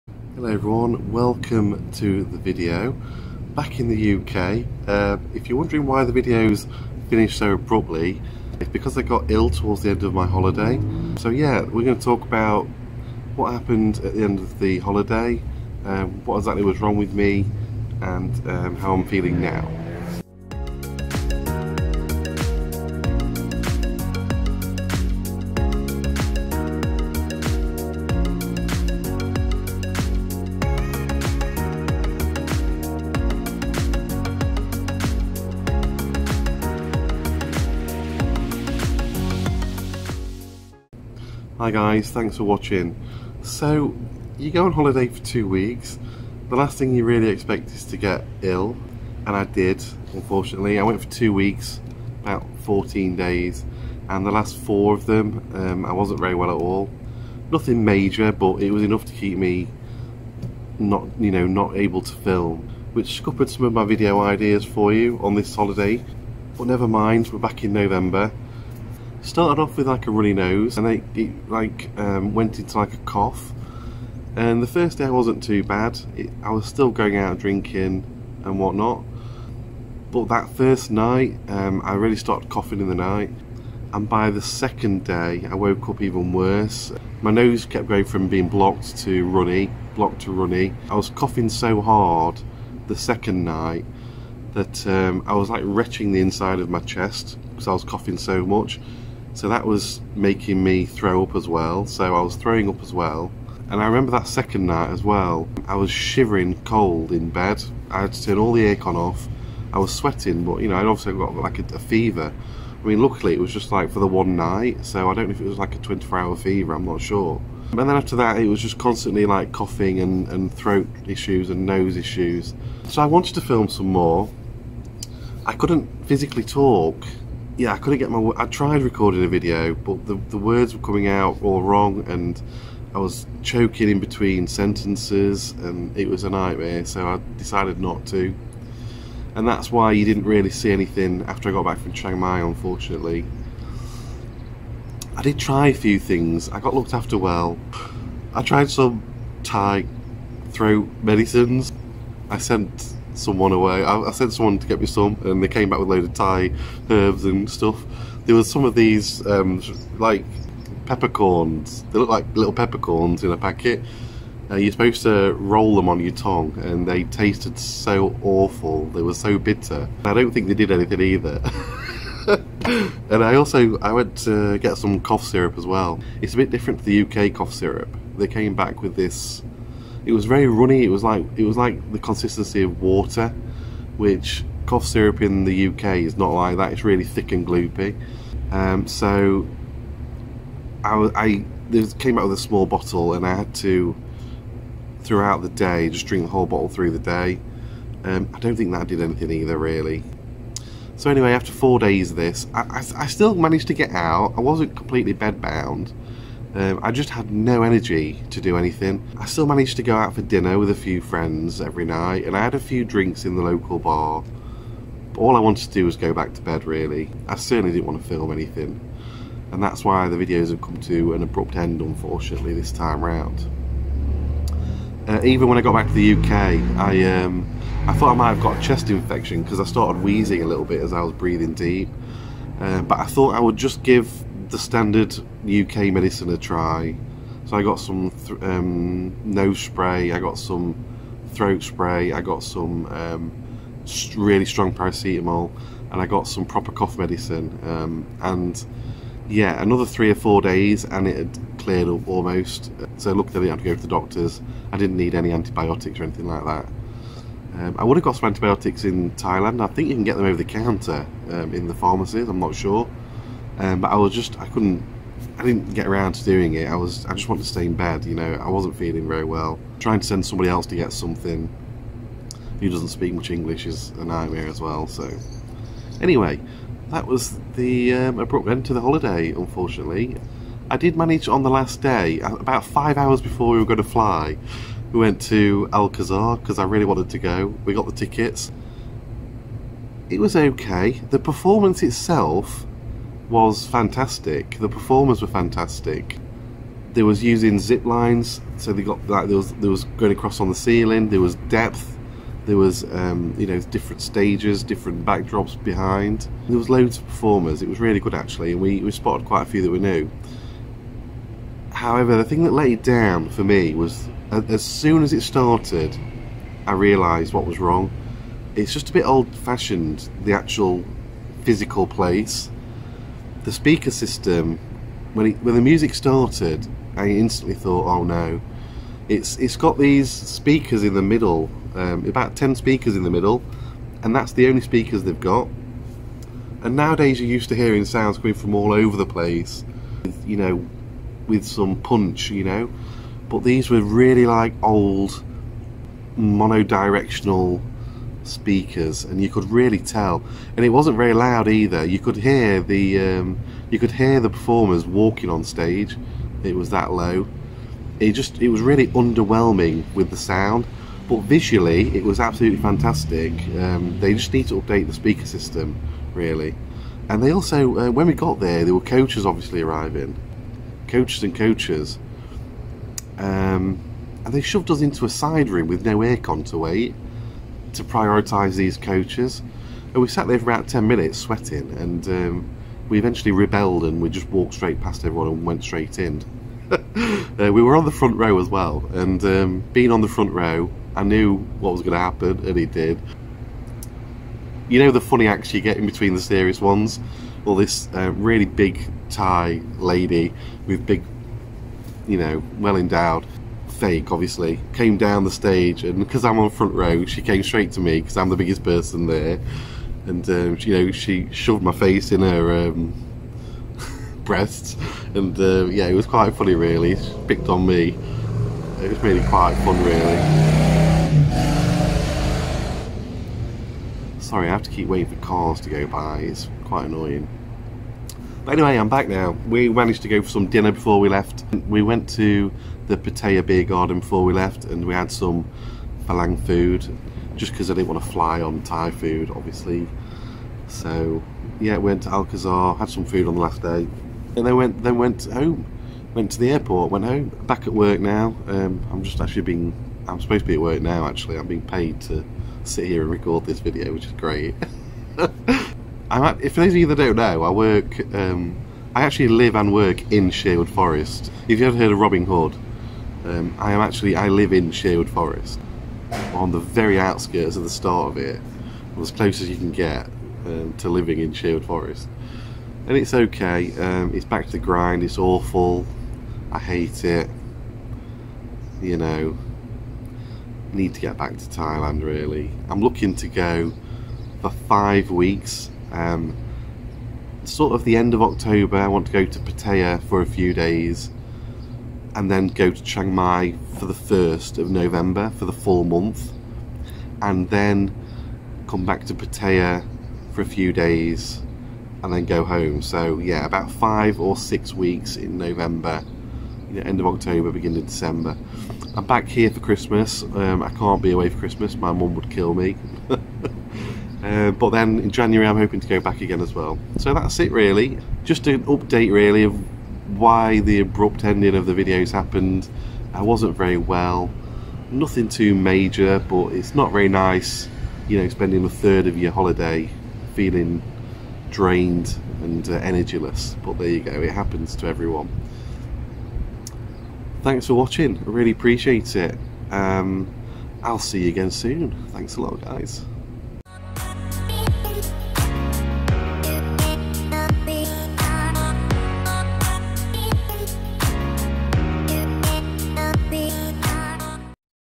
BEING SICK ON HOLIDAY IN THAILAND
Hello and welcome to the video,

Since arriving back in the UK, all things have gone back to normal. Looking at the end of the latest trip and how being sick made me feel